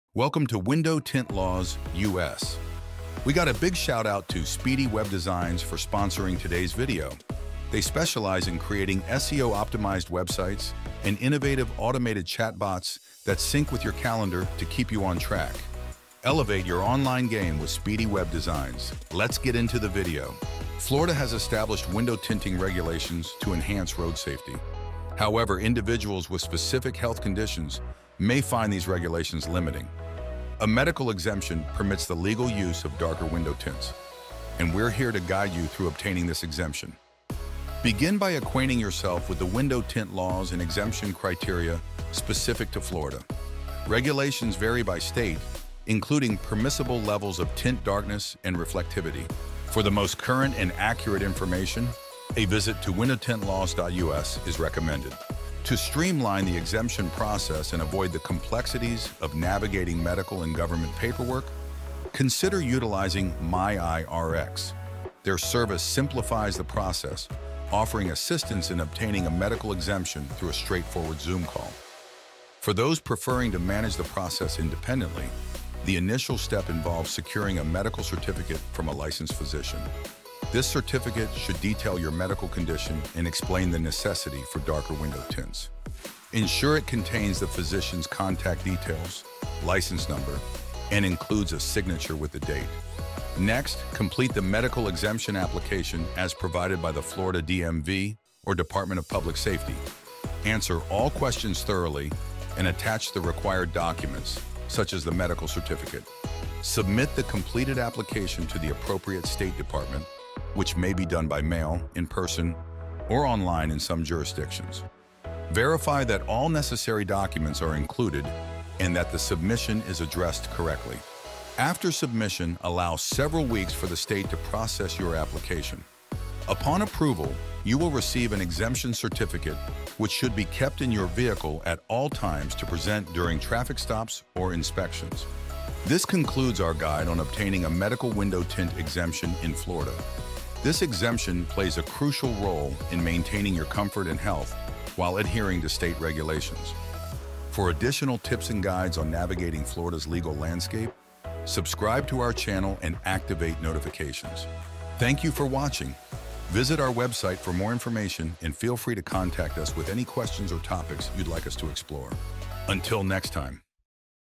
Florida Window Tint Medical Exemption 2024
Discover everything you need to know about Florida's window tint medical exemption for 2024 in this informative video! Learn how these regulations impact road safety and how individuals with specific health conditions can benefit from them. Don't miss out on this essential guide for staying compliant with the law.